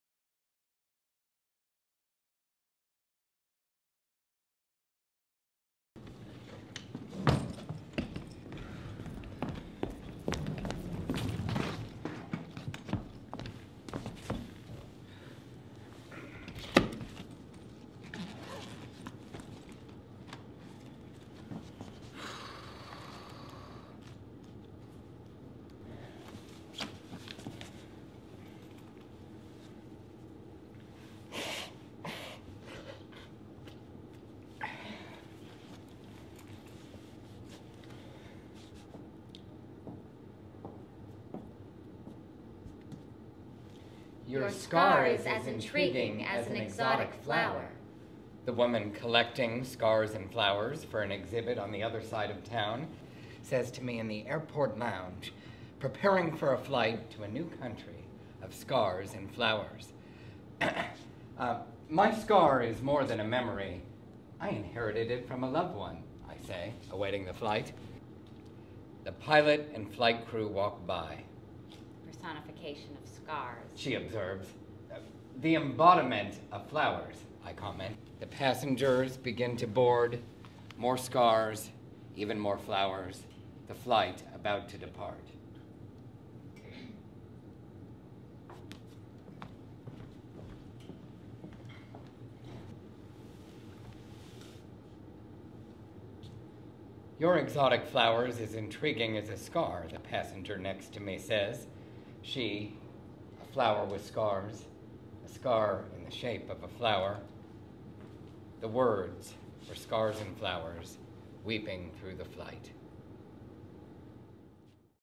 Unnatural History of Scars and Flowers by J.J. Steinfeld
The Unnatural History of Scars and Flowers by J.J. Steinfeld staged and directed by Gil Reyes and per performed by Andy Epstein and Sarah East. Segment produced by Job Lim Productions in Cooperation with Theatre 502. The Unnatural History of Scars and Flowers was publsihed in Fjords Arts and Literary Review Volume 1, Issue 2.